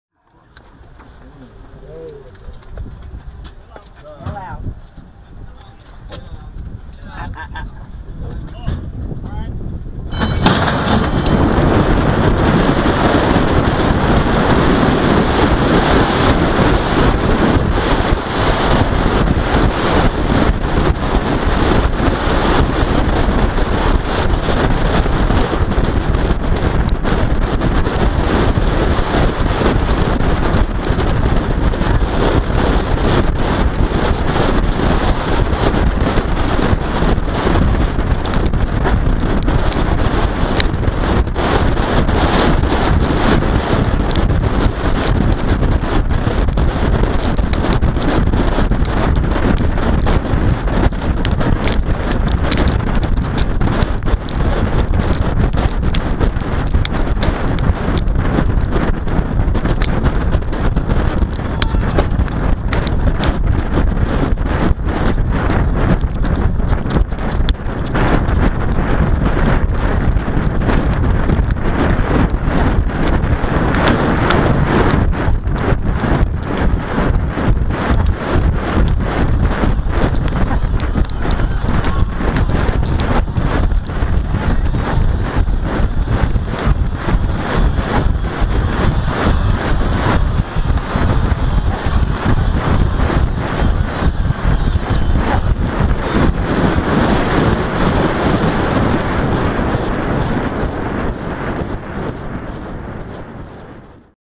Ride a Race with John Velazquez
Get a first hand view of what it is like to ride in a Thoroughbred race with this video that takes the viewer on board with Hall of Fame jockey John Velazquez on Greyfell, a 2-year-old colt who finished third in the third race at Keeneland Race Course on Oct. 5 - Fall Stars Saturday card.